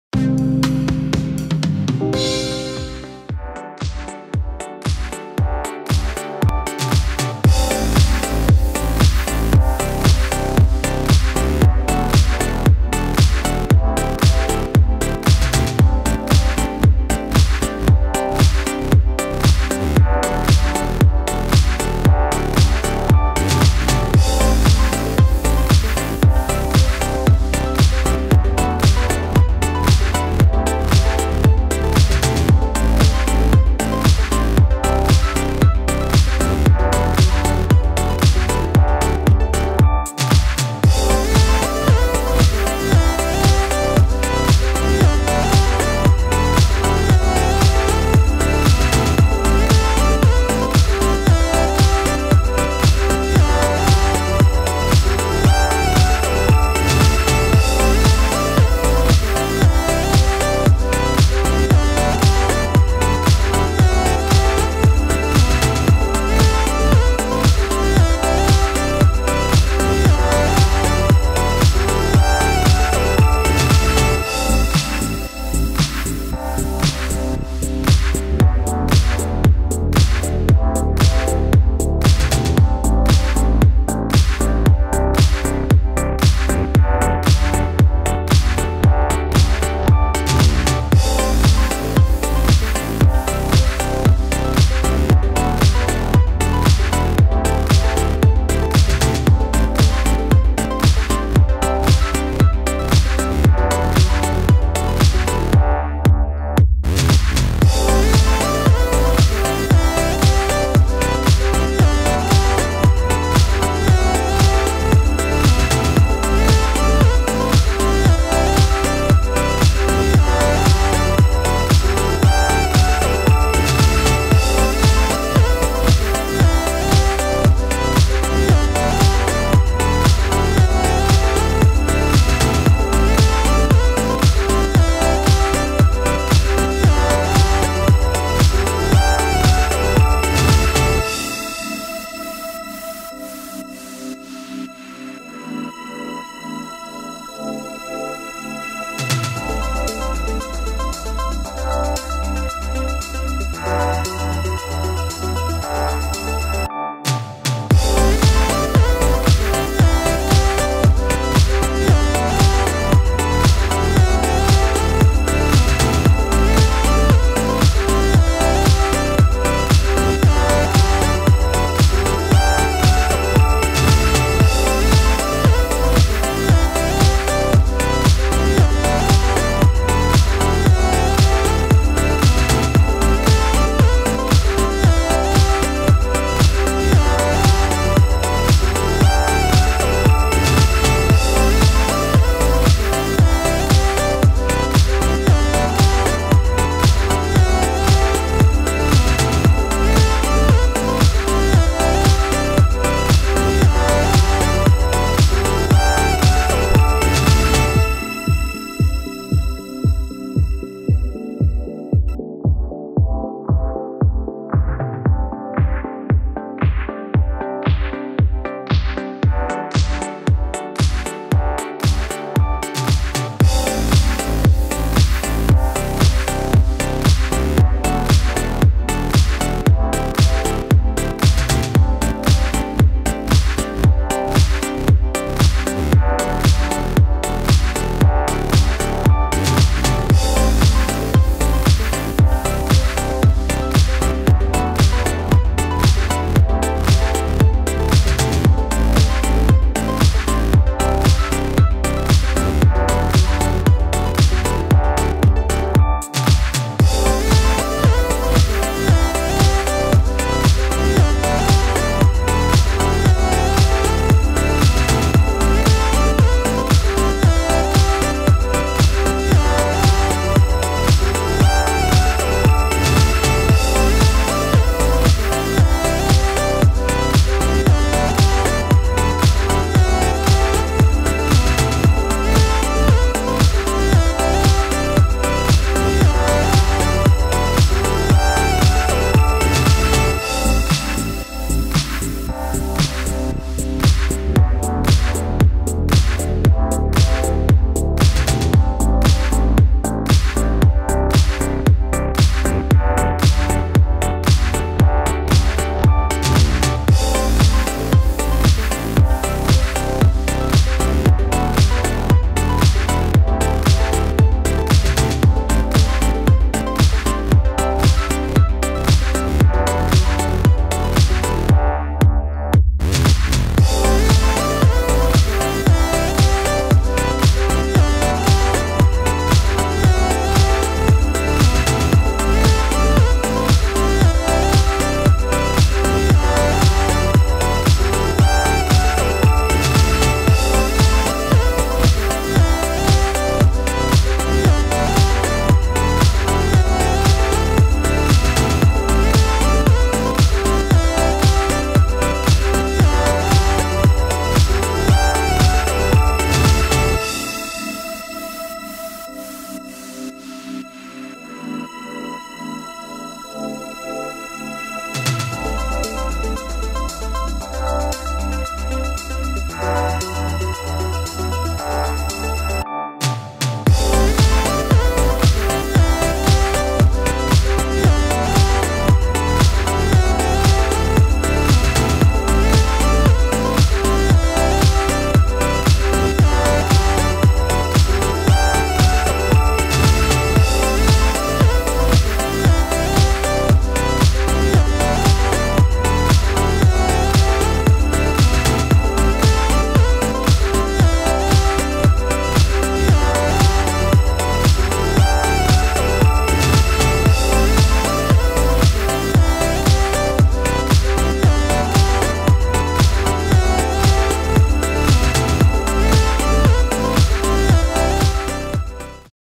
Сборка силового велотренажера OTO RowBike RB 1000
велотренажер OTO RowBike RB 1000 сборка и основные регулировки

Permissions beyond the scope of this license may be available